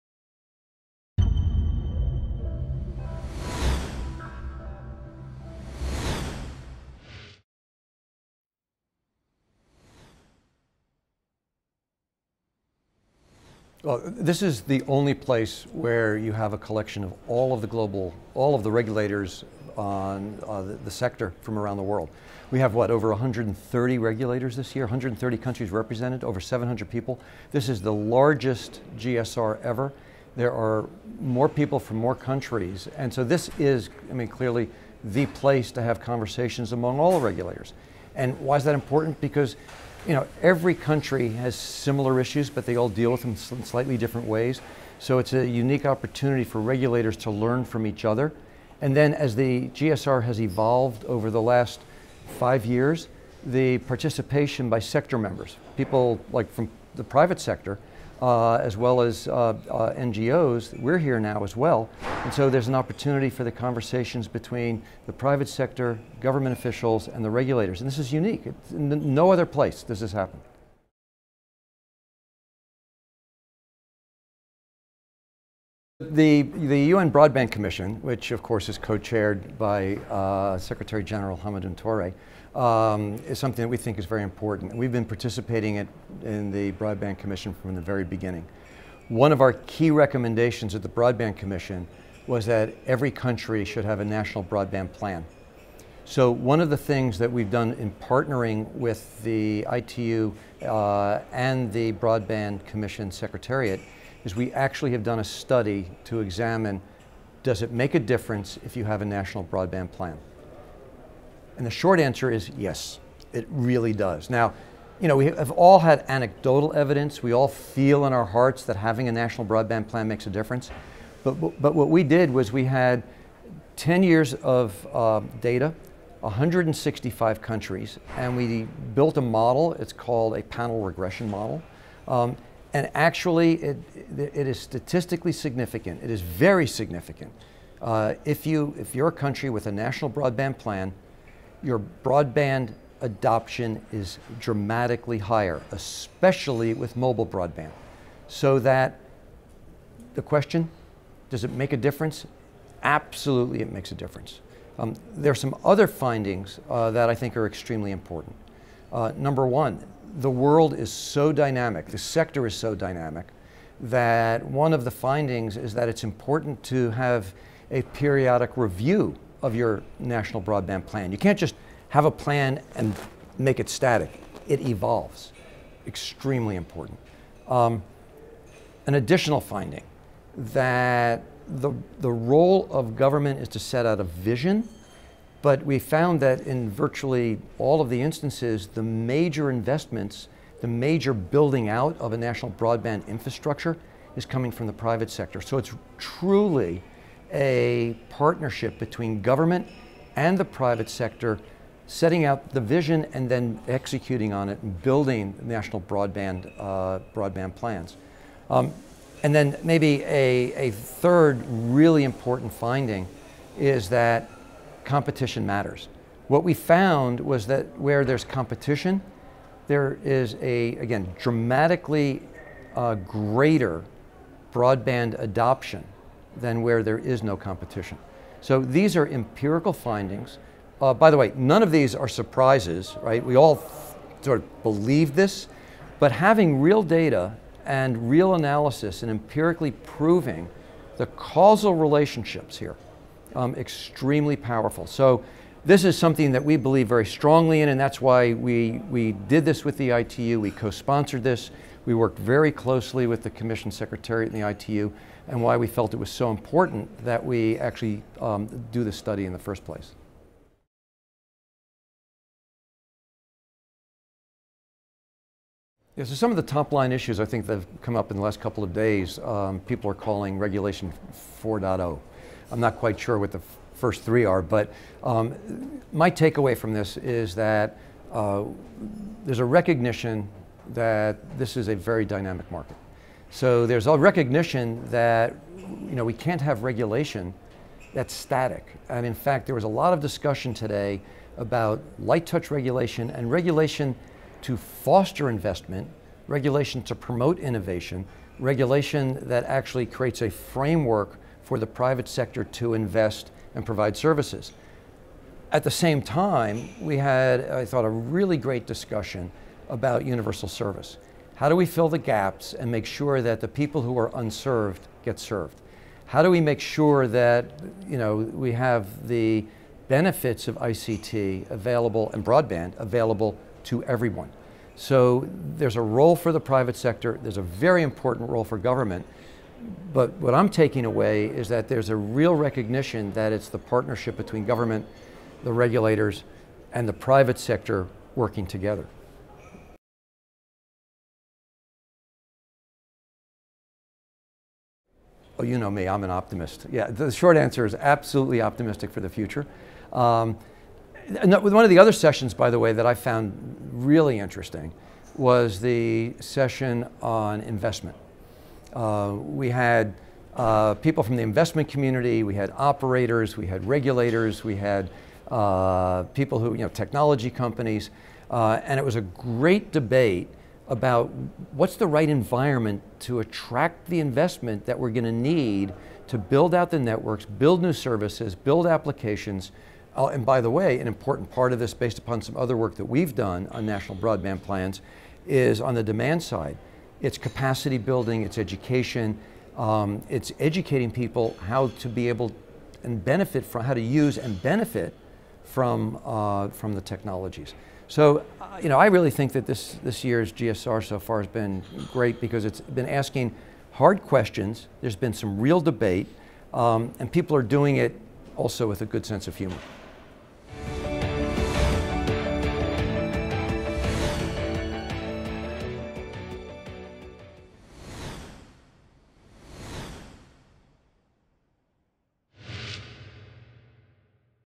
GSR13: Dr Robert Pepper, Vice-President, Global Technology Policy, CISCO
Interview with Dr Robert Pepper, Vice-President, Global Technology Policy, CISCO at the Global Symposium for Regulators 2013 (GSR13), Warsaw, Poland.